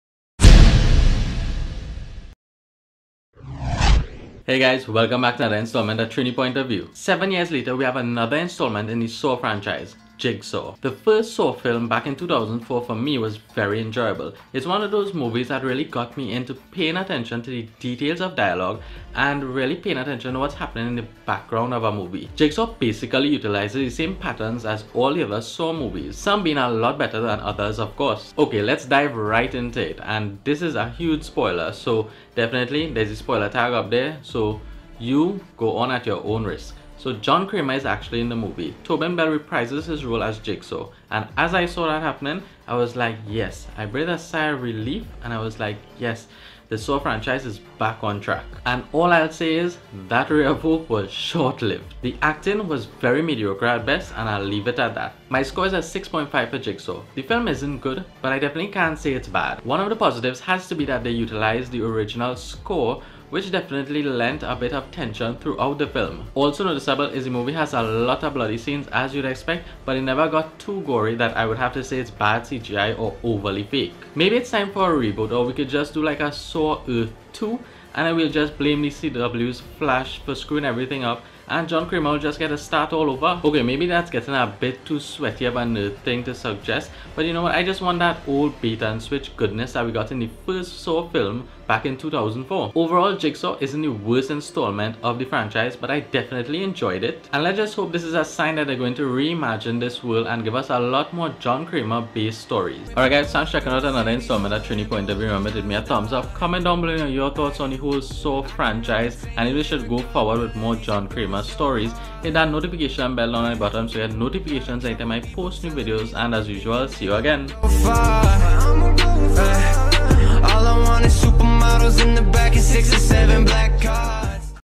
JIGSAW MOVIE REVIEW - SPOILERS
JIGSAW is directed by Michael and Peter Spierig, written by Josh Stolberg and Peter Goldfinger, and starring Matt Passmore, Callum Keith Rennie, Clé Bennett, and Hannah Emily Anderson. It is the eighth installment in the Saw franchise, and is considered a half prequel to the Saw franchise, picking up eleven years after the death of the eponymous Jigsaw killer.

SUBSRCIBE!!! SHARE!!! LIKE!!!